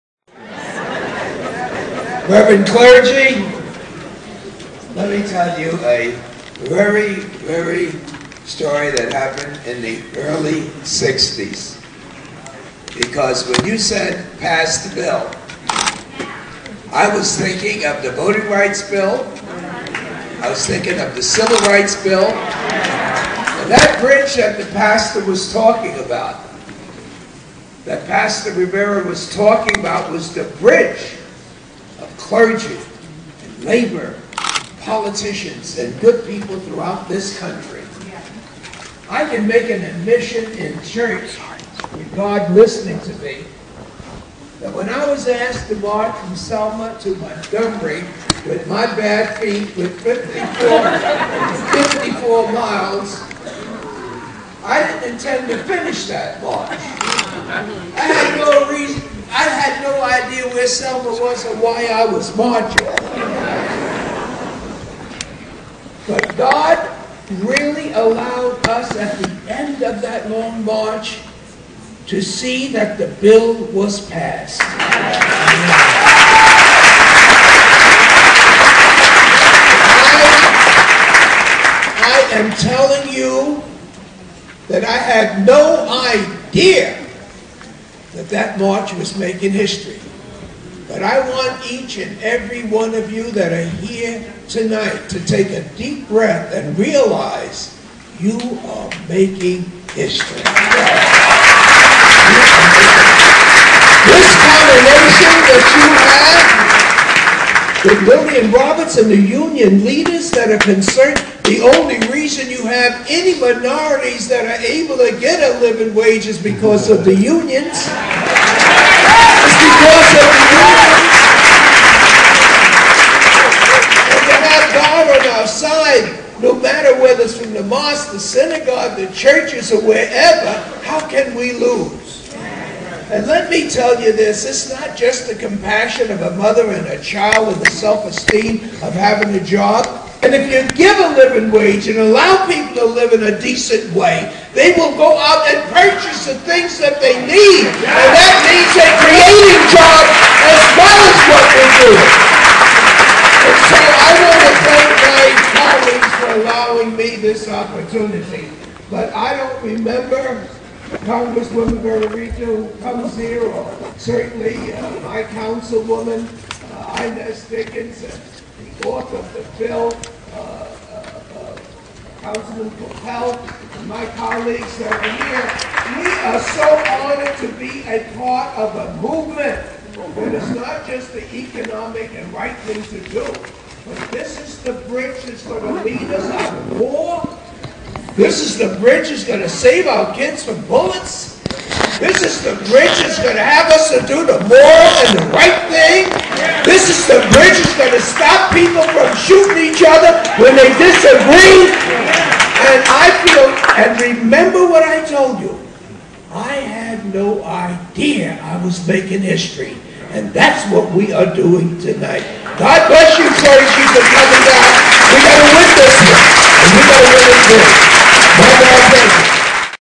Rangel Rallies For A Living Wage
Jan. 13 - Congressman Charles Rangel joins local residents, elected officials and members of the clergy in calling for wages that will allow working class New Yorkers stay in the city.